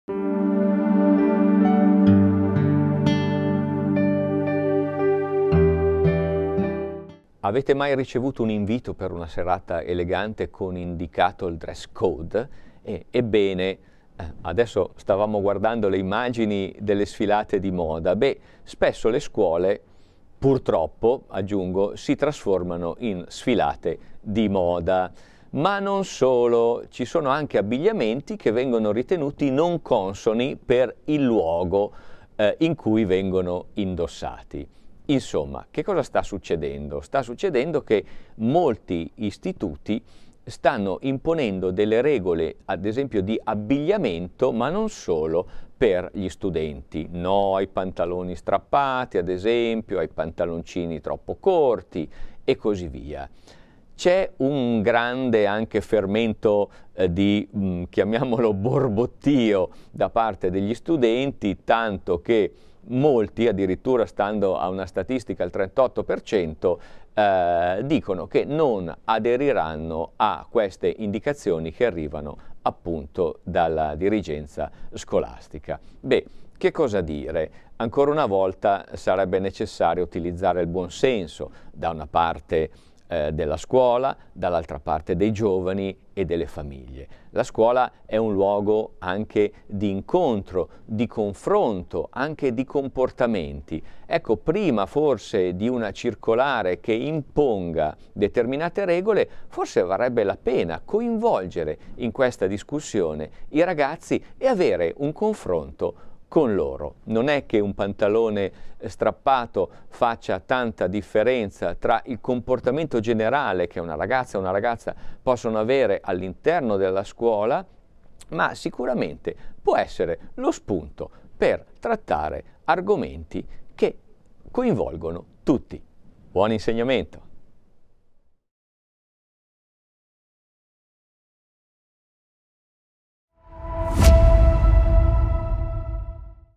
Dress code a scuola
Questa settimana la redazione di Scuola24 ci propone un argomento molto attuale e tanto dibattuto!

Esistono anche nella vostra scuola regole per l'abbigliamento degli alunni e, sopratutto, vengono rispettate?

Buona visione.